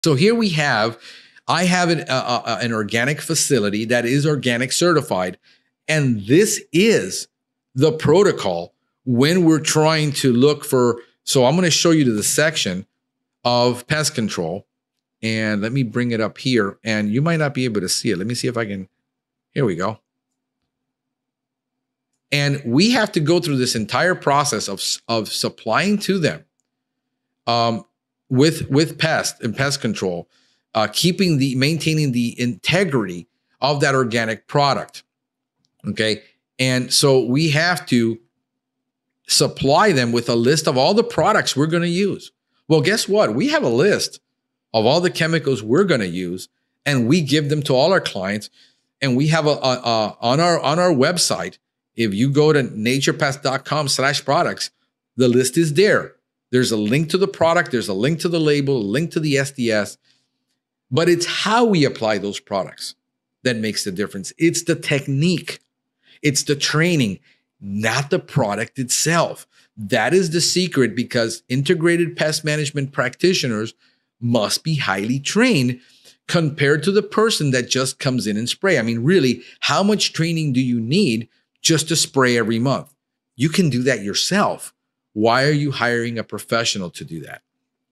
Master Organic Pest Control: Tips and Techniques from The Pestgeek!
**Welcome to Another Episode of the DIY Naturepest Podcast!**

Join holistic pest control management specialist Franklin 'The Pestgeek' Hernandez as he unveils the secrets to effective organic pest control. In this episode, Franklin shares his expert insights on maintaining pest-free environments using eco-friendly and sustainable methods. Whether you're dealing with ants, roaches, fleas, or ticks, Franklin's strategies will help you keep your home and garden safe and pest-free.

### Episode Highlights:
- **Organic Pest Control Protocols:** Franklin discusses the detailed protocols for maintaining organic certification and ensuring the integrity of organic products.
- **Integrated Pest Management (IPM):** Learn how IPM techniques are crucial for effective pest control. Franklin emphasizes the importance of training and technique over merely using products.
- **Product Transparency:** Discover how NaturePest provides clients with a comprehensive list of all products used, including links to labels and safety data sheets (SDS).
- **DIY Pest Control Tips:** Franklin offers practical advice for those who prefer to manage pest control on their own, highlighting the differences between professional and DIY approaches.

Don't miss this episode packed with valuable information on achieving effective and sustainable pest control. Equip yourself with the knowledge to protect your home and family while being environmentally conscious.